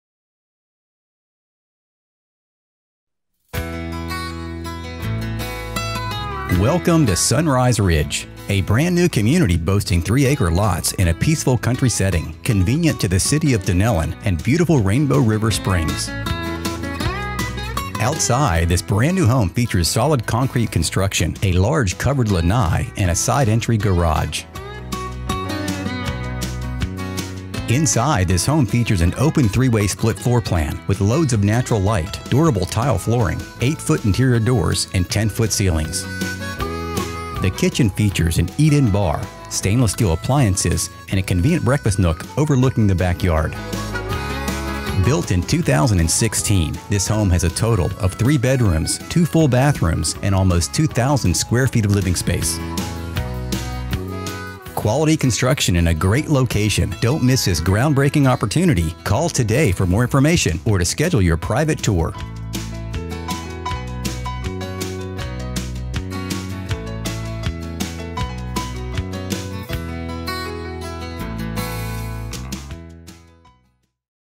Sunrise Ridge - Dunnellon, Florida
QUALITY NEW CONSTRUCTION! Beautiful 3BD/2BA concrete block homes available on scenic 3 acre lots in the quiet, deed protected subdivision of Sunrise Ridge. Located just minutes from Rainbow River and Rainbow Springs State Park, some of the homes many features include:
• Concrete block construction/stucco exterior
• 8 ft. interior doors/9 ft. ceilings
• Stainless steel appliances in kitchen
• Ceramic tile throughout
• Carpeted bedrooms